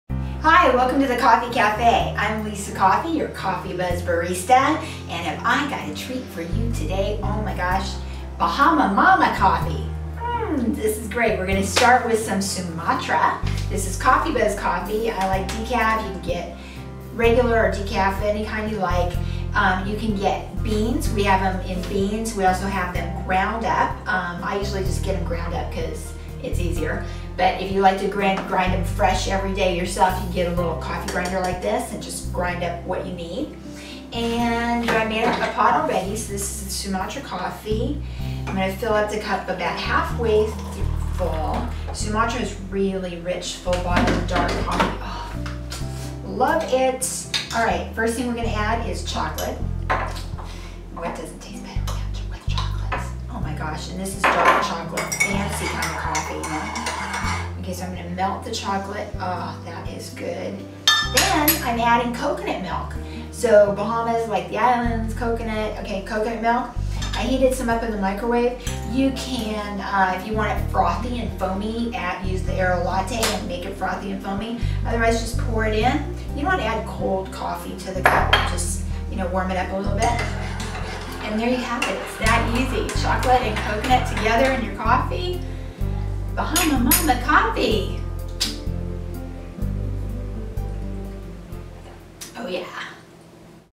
Coffey Cafe: Bahama Mama Coffee
Lissa Coffey makes a Bahama Mama Coffee drink using CoffeyBuzz coffee. This is a delicious coffee that can be made with either regular or decaffeinated coffee.